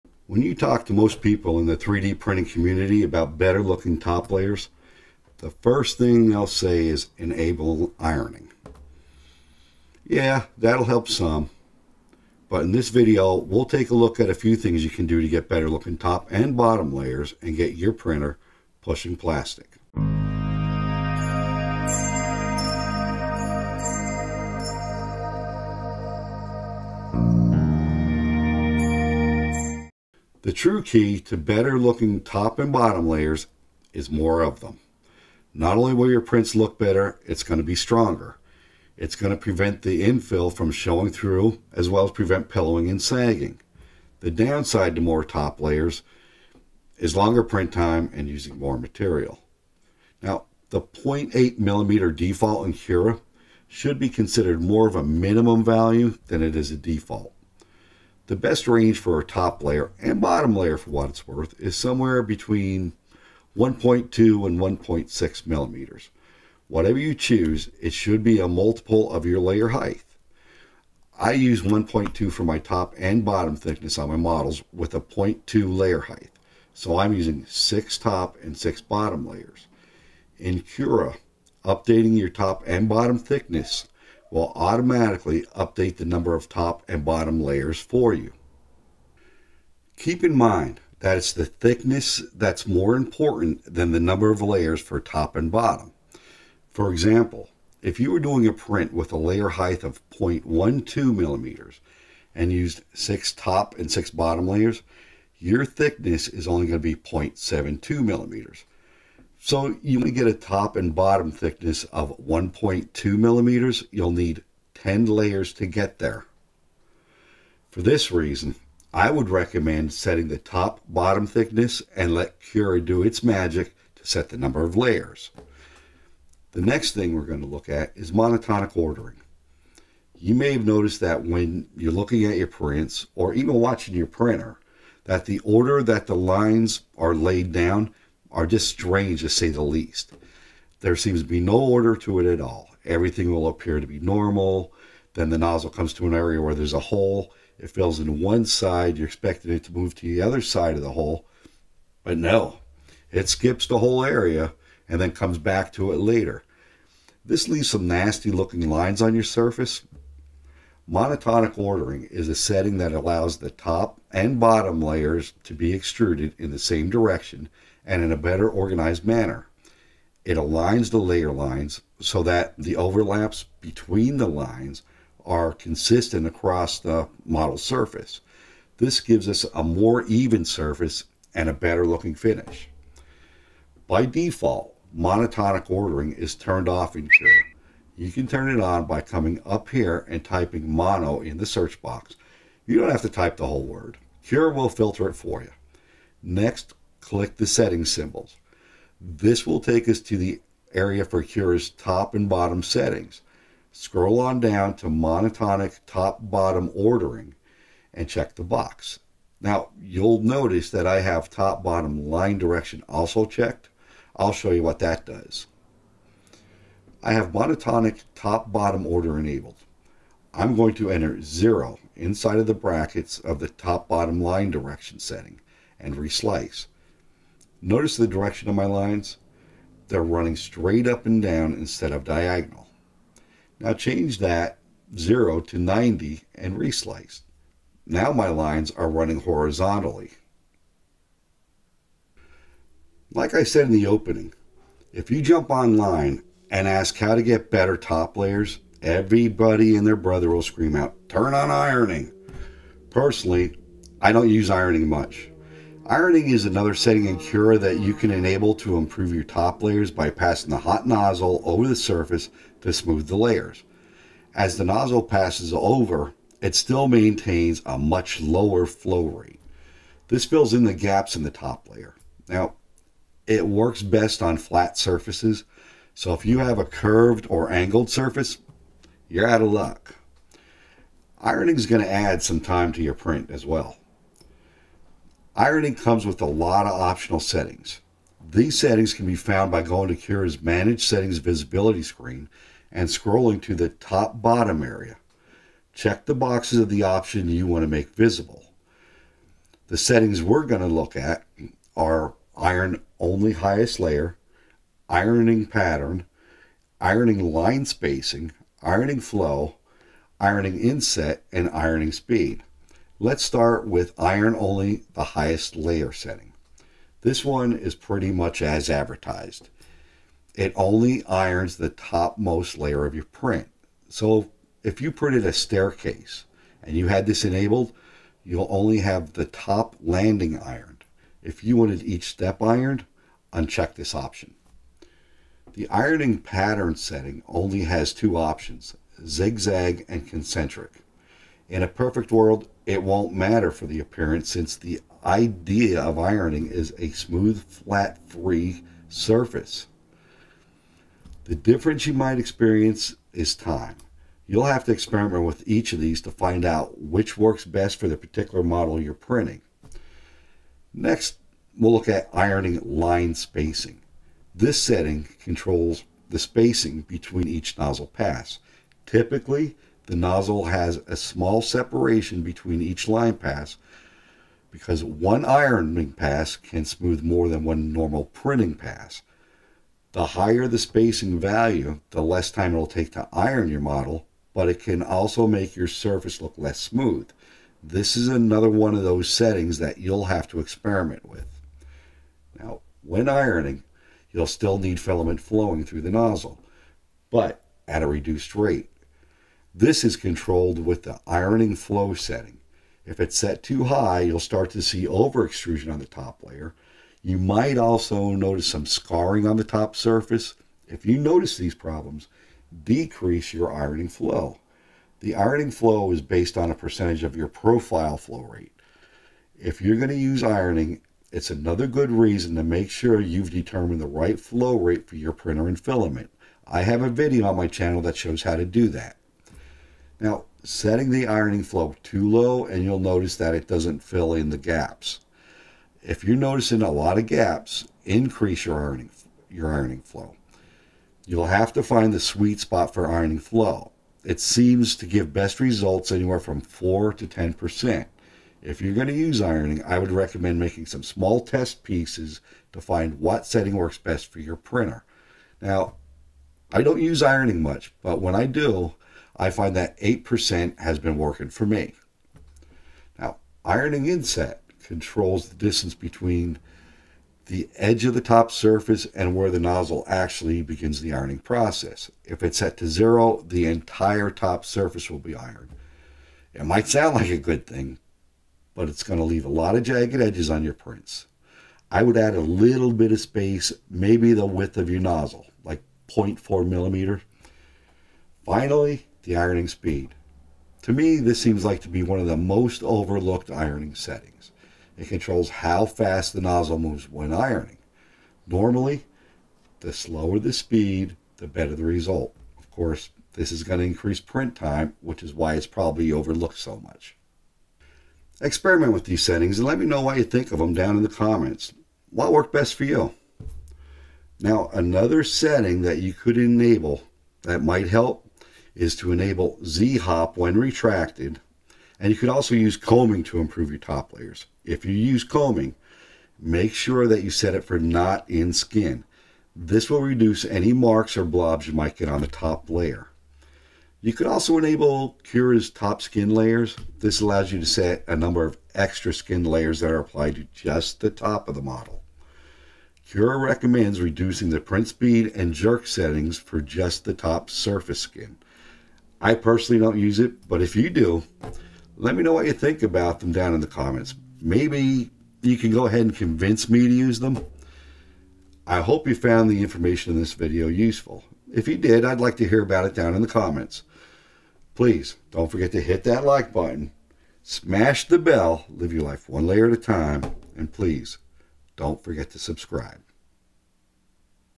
Understanding Top and Bottom Layers: The Key to Perfecting Your 3D Prints
Take your 3D printed models to the next level with amazing results with better looking top lays.